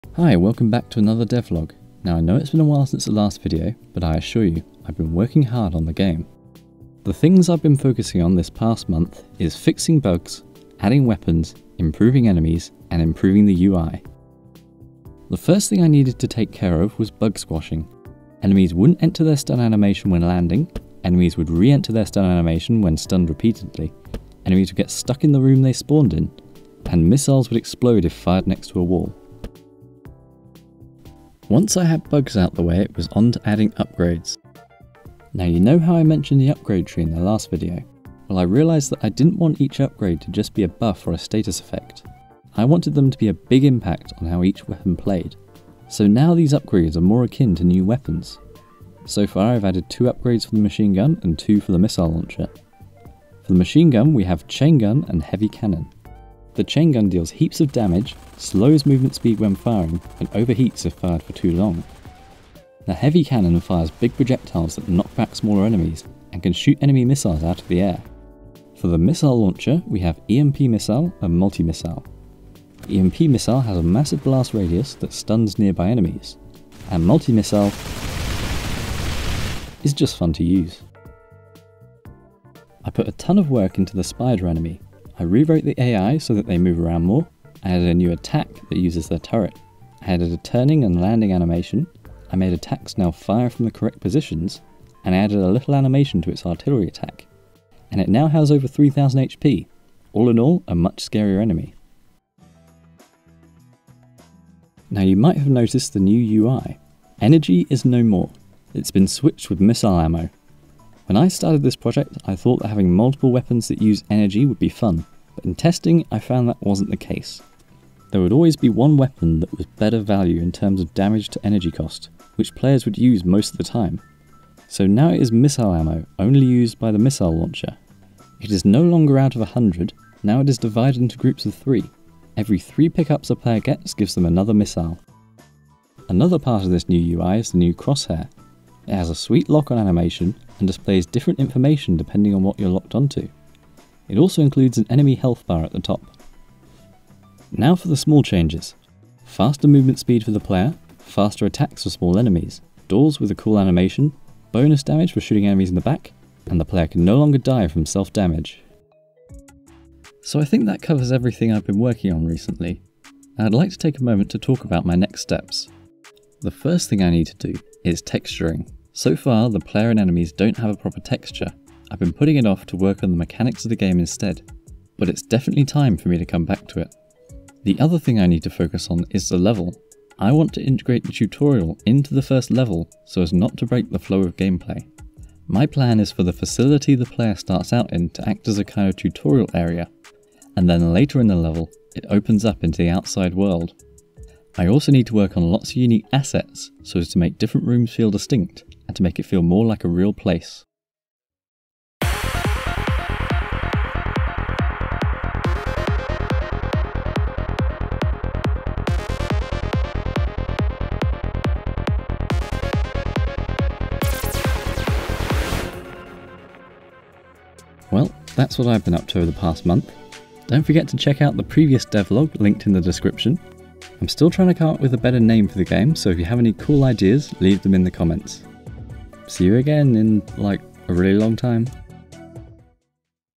Indie Game Devlog #2 - New Upgrades And UI
This is a quick indie game devlog for a twin stick shooter I am making using Unreal Engine. This video covers the latest changes which mostly consist of bug fixes, a new UI, new upgrades and enemy improvements.